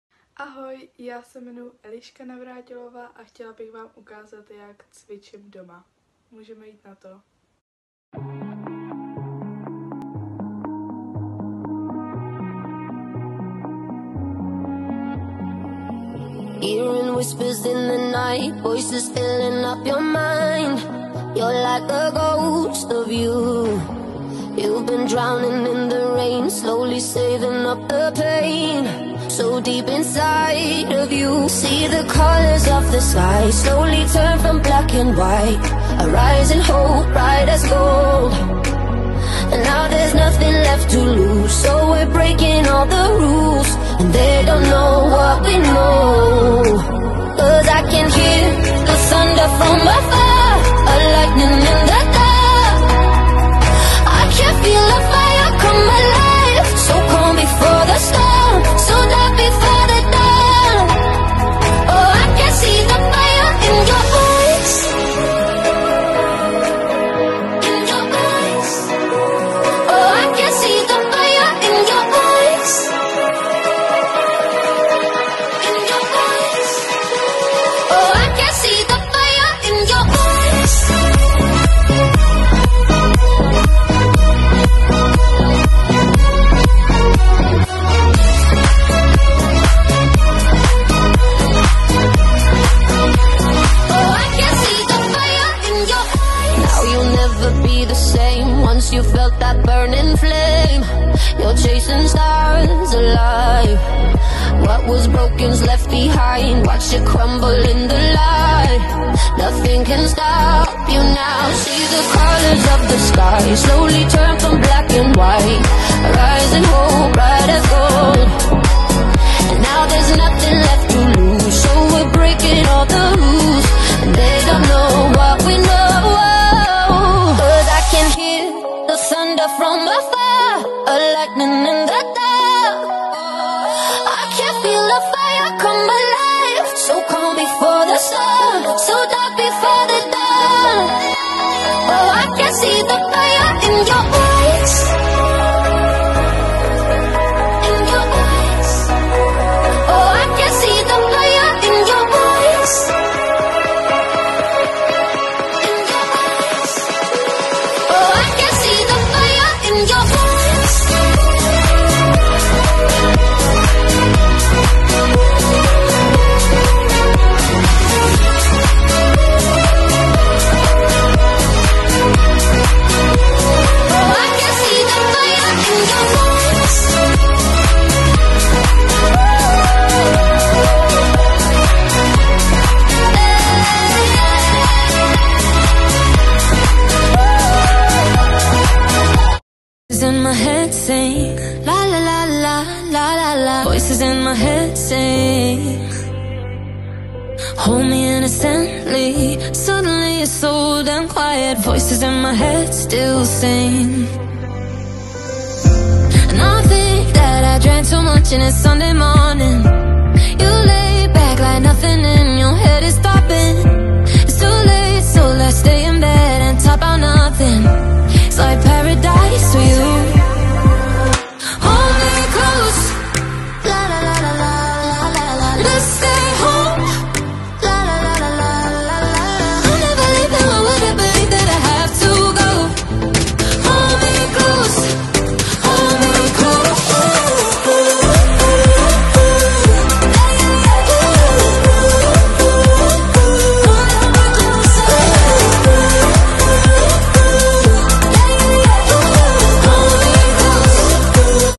Cvičení s Eliškou Navrátilovou
Video v rámci programu SPOLEČNÍK - projekt pro mladé hasiče v době vyhlášení nouzového stavu.